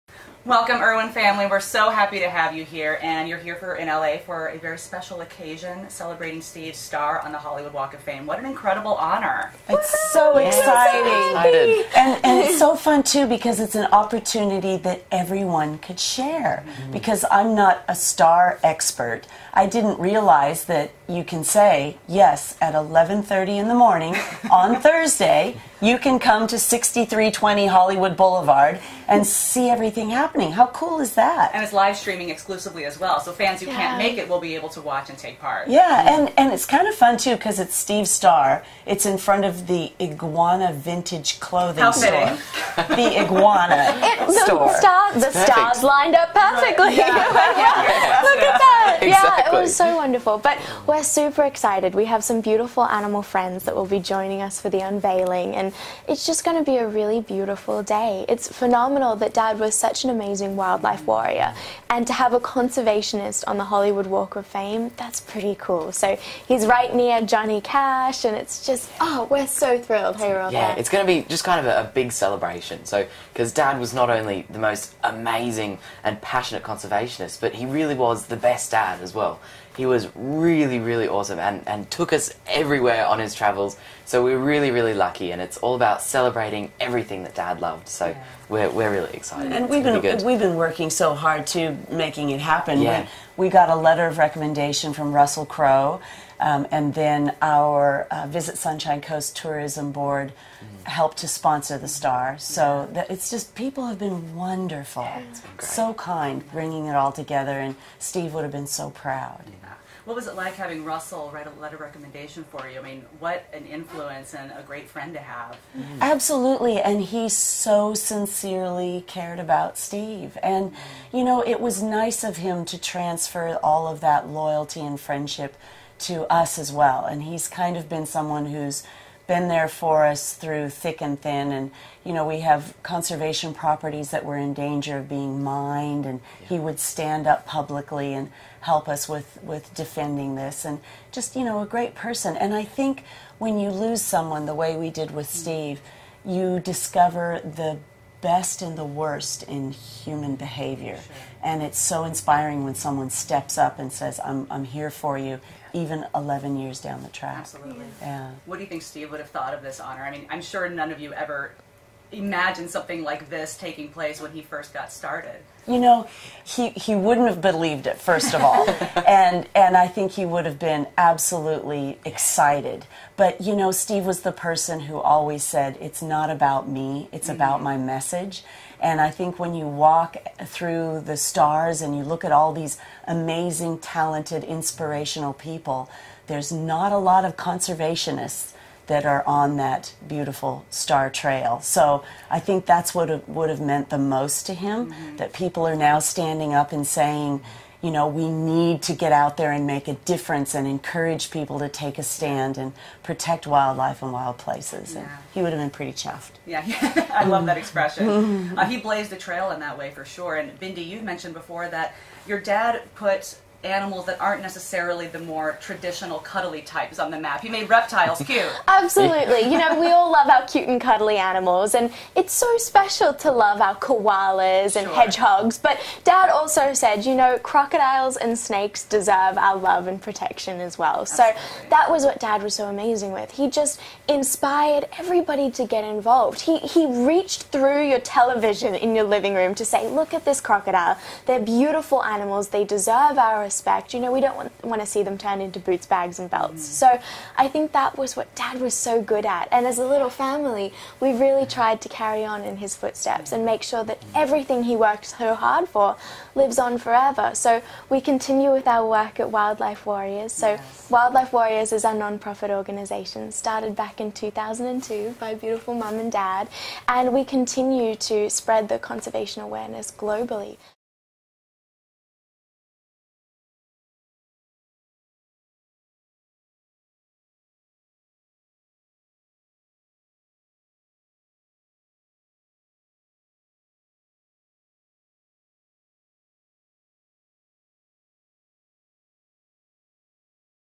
Bindi, Robert & Terri Irwin Share Their Excitement For Steve Irwin's Walk Of Fame Star | Access
Crikey, what an incredible honor! Bindi, Robert and Terri Irwin sit with Access and share their excitement about the late Steve Irwin receiving a star on the Hollywood Walk of Fame. How did family friend Russell Crowe step in to help? Plus, what would Steve think about being immortalized in such an iconic way?

Bindi, Robert & Terri Irwin Share Their Excitement For The Late Steve Irwin's Walk Of Fame Star | Access